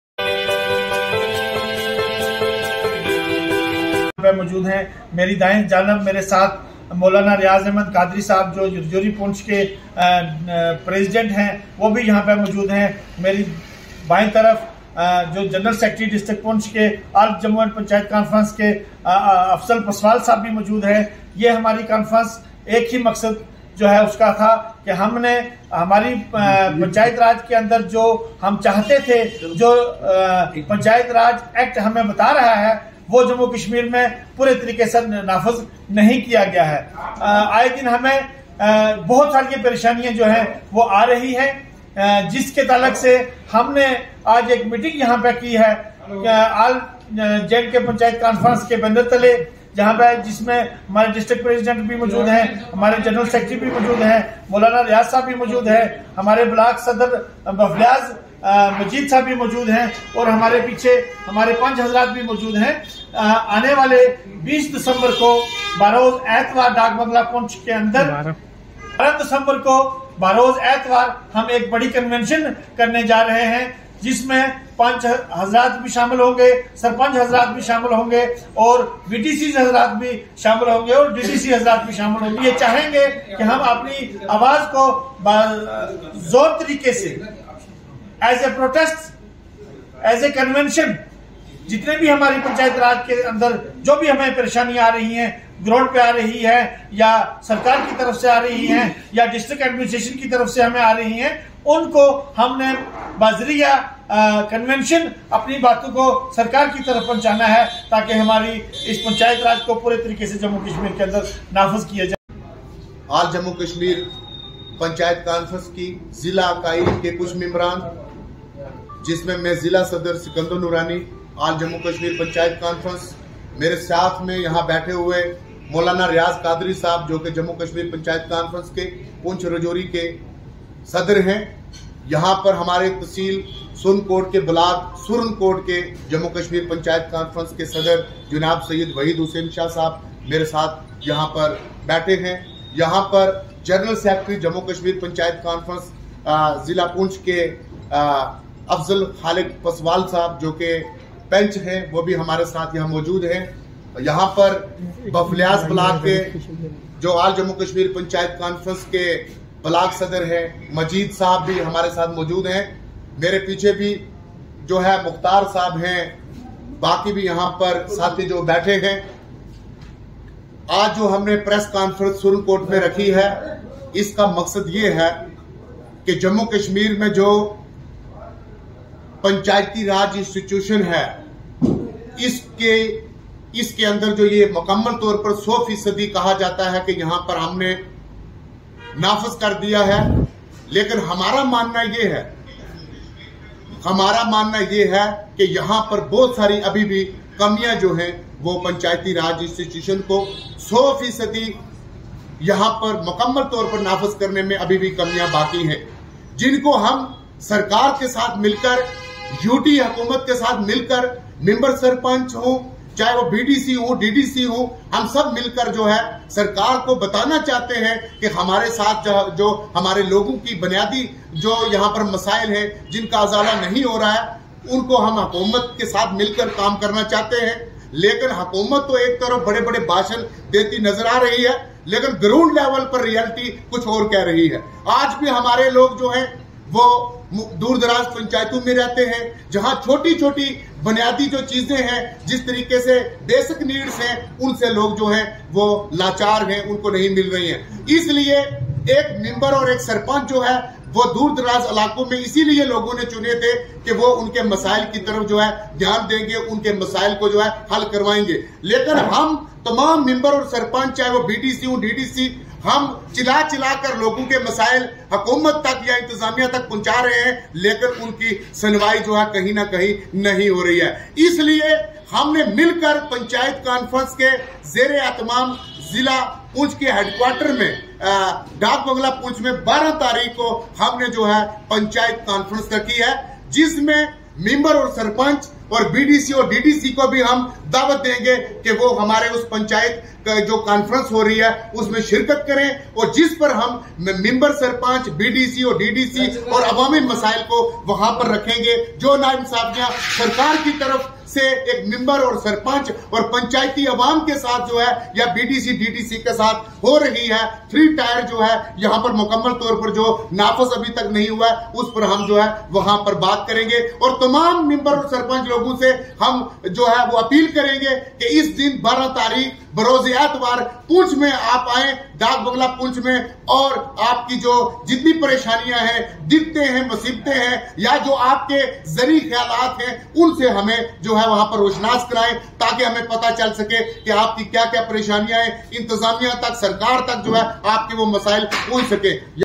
All jammu and kashmir panchayat conference Members held a press conference at Surankote
*All jammu and kashmir panchayat conference Members held a press conference at Surankote And appeals to All Panch Sarpanch BDC and DDC to Attend Panchayat Convention On 12th December (Sunday) At 10Am At Dhakbanglow Poonch. All Jammu Kashmir Panchayat Conference District President Poonch Sakander Nourani,Waheed Hussain Shah Block President Surankote, General Secretary Poonch Afzal Khaliq Paswal,Moulana Raiz Qadari President Poonch Rajouri Block President Bufliaz Abdul Majeed and others participated*